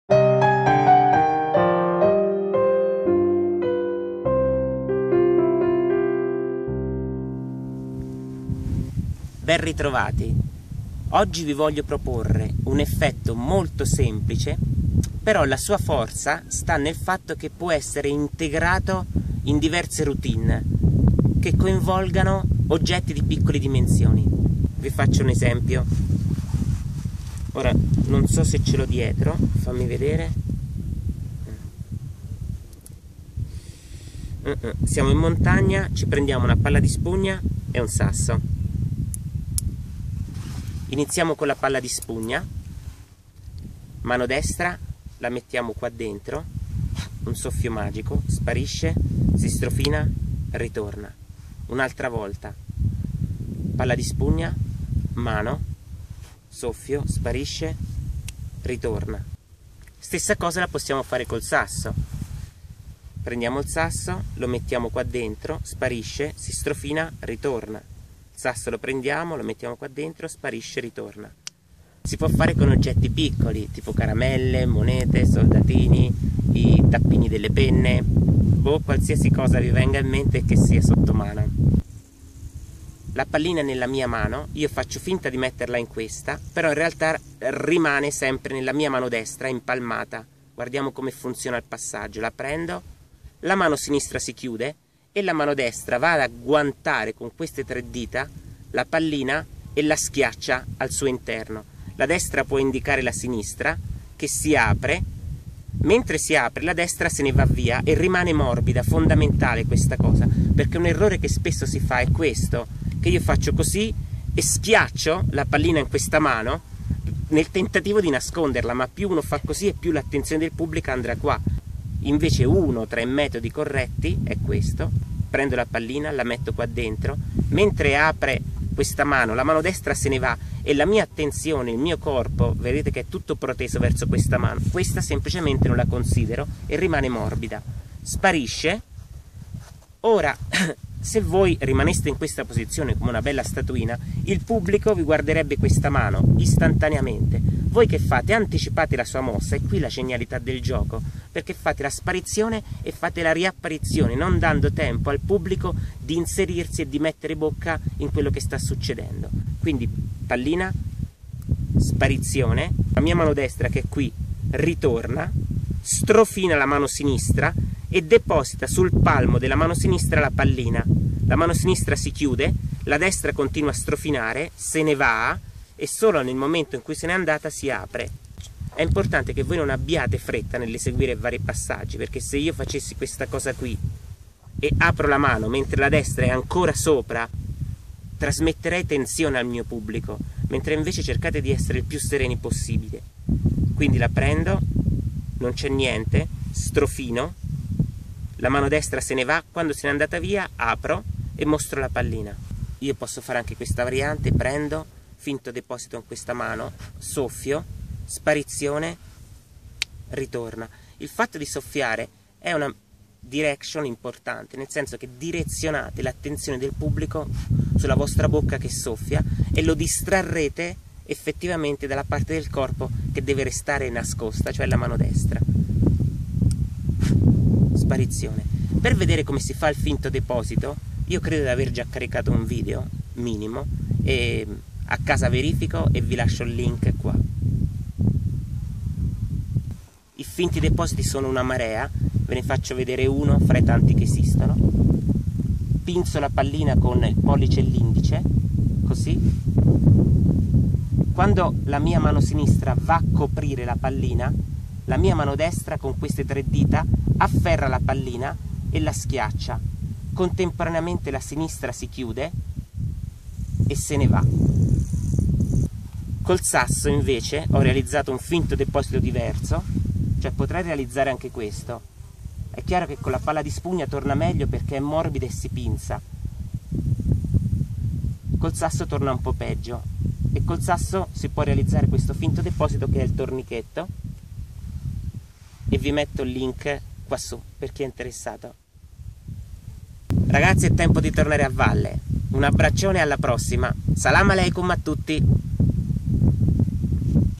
Come Far Sparire ed Apparire Oggetti di Piccole Dimensioni - TUTORIAL
Tutto ciò che vuoi è dall’altra parte della paura
Jack Canfield